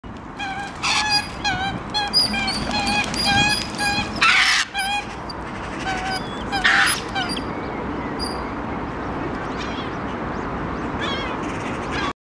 heron coot showdown.MOV
The coot parents do their best to chase off the heron